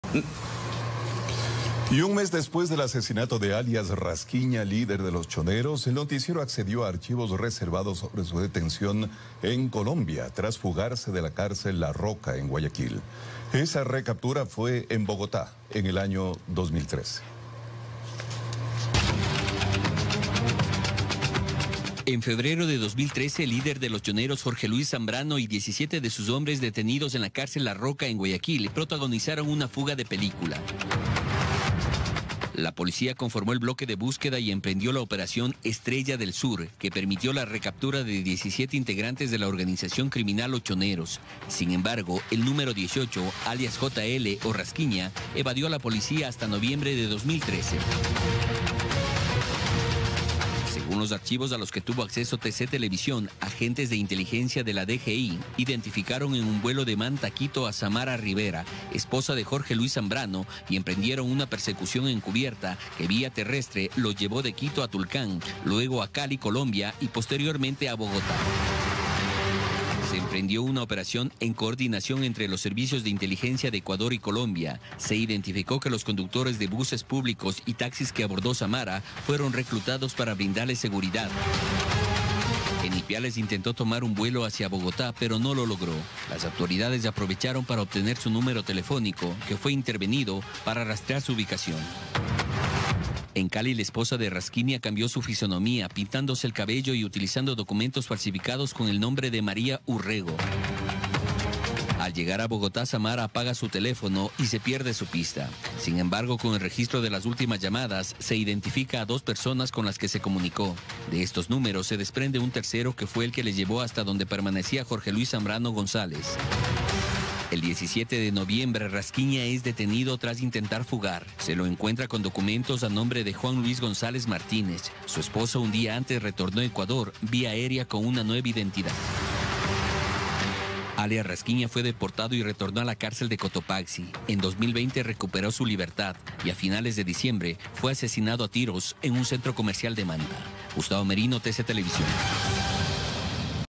¿Cómo se capturó a Rasquiña, líder de los Choneros, en Colombia en 2013?
UNA OPERACION COMBINADA ENTRE EQUIPOS DE INTELIGENCIA DE ECUADOR Y DE COLOMBIA PERMITIO EN 2013 LA CAPTURA EN BOGOTA DEL LIDER DE LOS CHONEROS JORGE LUIS ZAMBRANO ALIAS RASQUIÑA Y QUIEN FUE ASESINADO A FINALES DE 2020 EN MANTA. EL NOTICIERO TUVO ACCESO  A LOS ARCHIVOS RESERVADOS DE COMO SE REALIZÓ ESTA OPERACIÓN DE RECAPTURA.